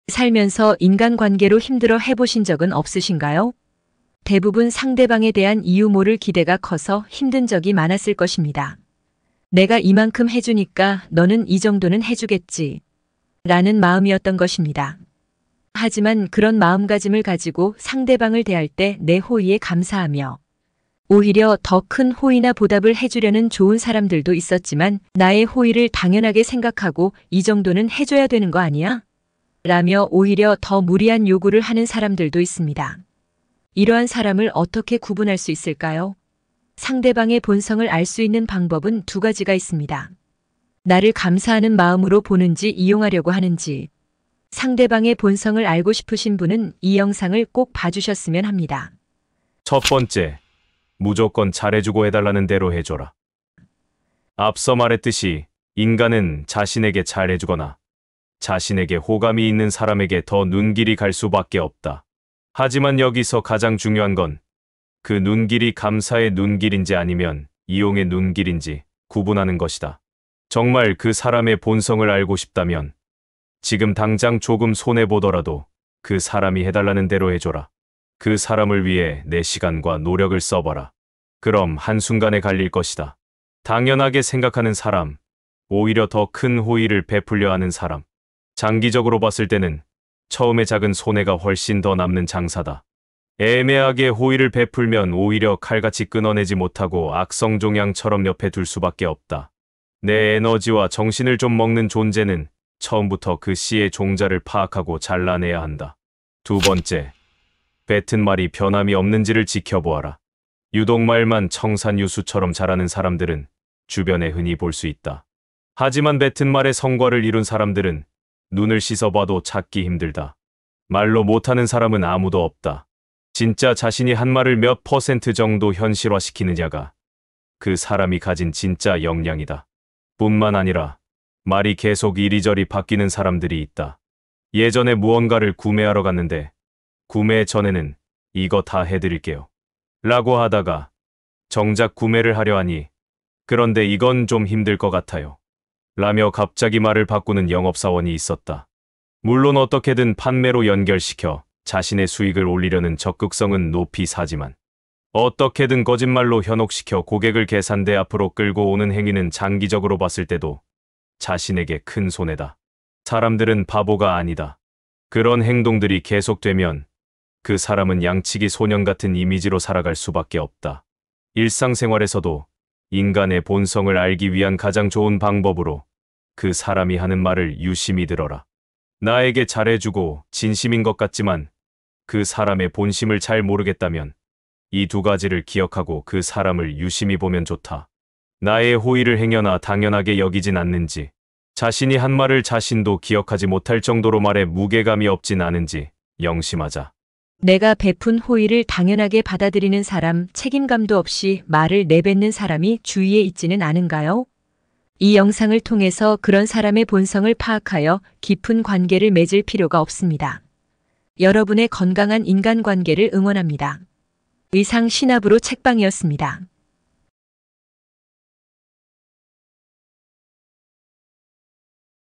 인간의 본성을 알 수 있는 좋은 방법/무례한 사람에게 휘둘리지 않는 법/인간관계/오디오북
더 이상 무례한 사람에게 휘둘릴 필요도 없고 상처받을 필요도 없다. 다음 영상은
무례함을 저지르는 상대방을 단호하게 차단하고 스스로를 지키는 것이 최고의 관계 처방전이다.
오디오북처럼 편하게 들으셔도 됩니다.

참고문헌
무례한 사람에게 휘둘리지 않는 법
지은이 – 정재훈
펴낸 곳 – 바이오엠 스튜디오
남들이 나를 함부로 하지 못하게 하라
무옌거 지음/최인애 옮김
펴낸 곳 – 쌤앤파커스
이 외에 인간관계에 대한 인사이트를 더하여 만든 2차 저작물입니다.